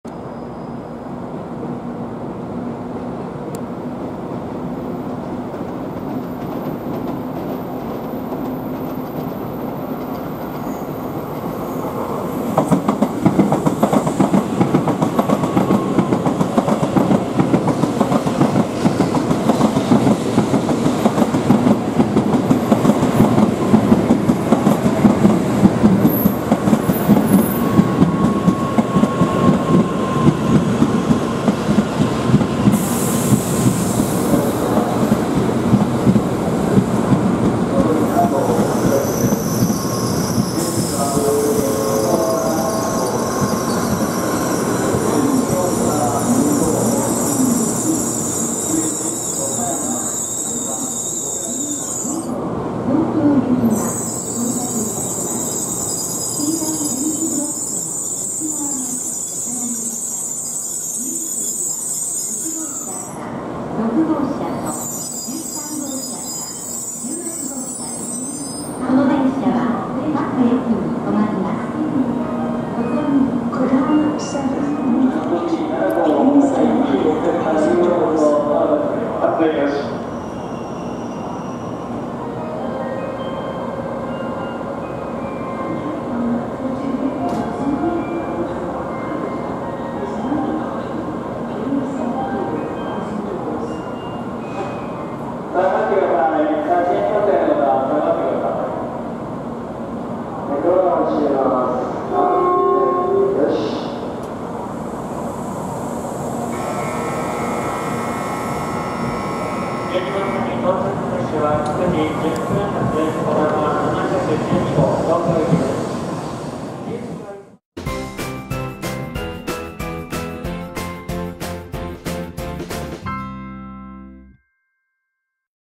ショート動画☆やっぱりいいよね！東海道新幹線。久しぶりに出会ったら！
東海道新幹線京都駅です。
久しぶりに出会ったらサプライズもらいました！